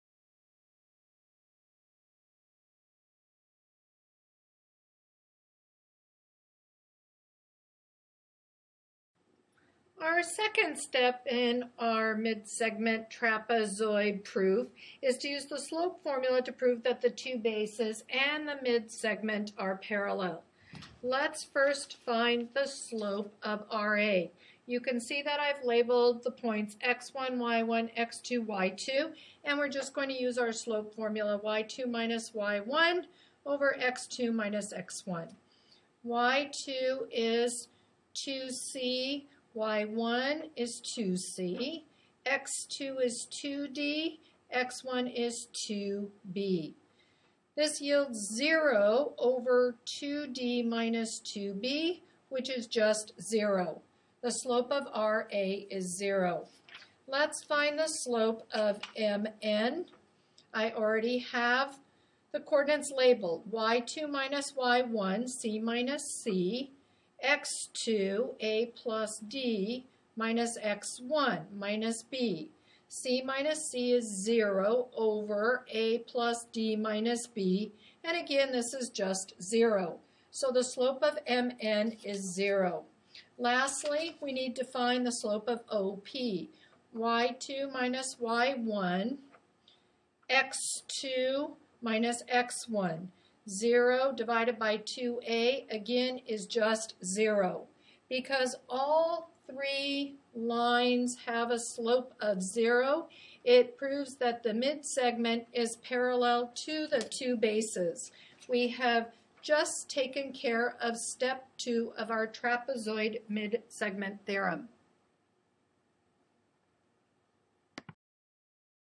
Coordinate Slope
Using variable coordinates and the slope formula to find the slope of the midsegment and 2 bases of a trapezoid (2 of 4).